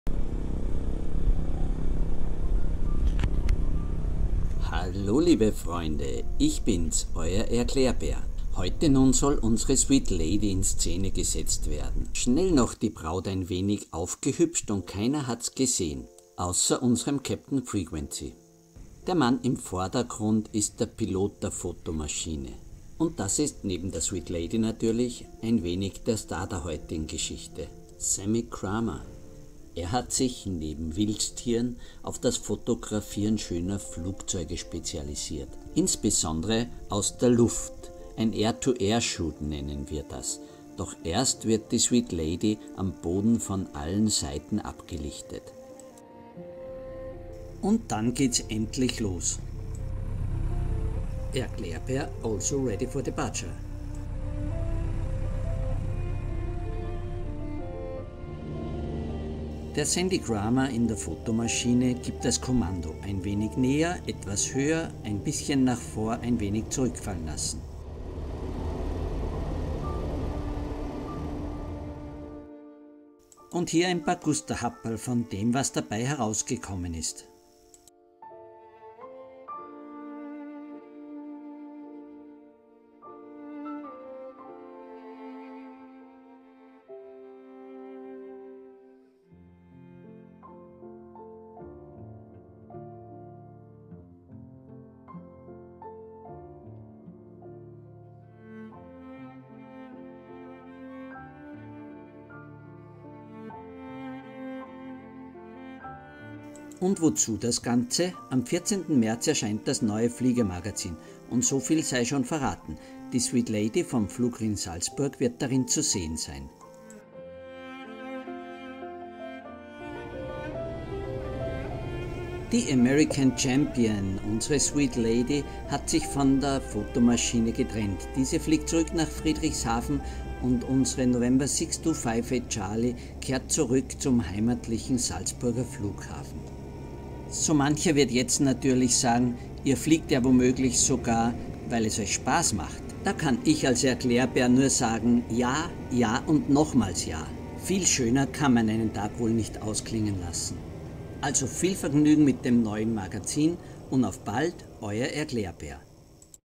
shooting the sweet lady
unsere sweet Lady ist schon ein besonderes Stück Flugzeuggeschichte. Die Geschichte unserer American Champion würde wohl auch eine breitere Leserschaft interessieren, und ein Spornrad im Verein wird ohnehin immer seltener. So sandten wir die die Story unserer Oldtimerin an das Fliegermagazin, nur mit den Bildern war man nicht ganz zufrieden. Für so ein Glanzstück wollte man den besten Air to Air Fotografen, den das Fliegermagazin aufbieten konnte, Sandy Kramer.
Erst vor Kurzem waren Fotos von ihm,  welche Flugzeuge vor der Golden Gate zeigten, Thema in diesem Flugjournal. Nur, dass der zur Zeit in Kanada weilte. so wurde er kurzerhand nach Europa beordert, flog von Friedrichshafen aus mit einer Fotomaschine zu uns, um ein kleines Wetterfenster bestmöglich zu nutzen. Beim "making of" durfte der ErklärbAIR dabei sein, und die N6258C endgültig zur Diva erklären.
Am 14. März soll dieses Magazin dann erscheinen, heißt, dass es bei meiner Trafik am Mittwoch zur Abholung bereit liegt.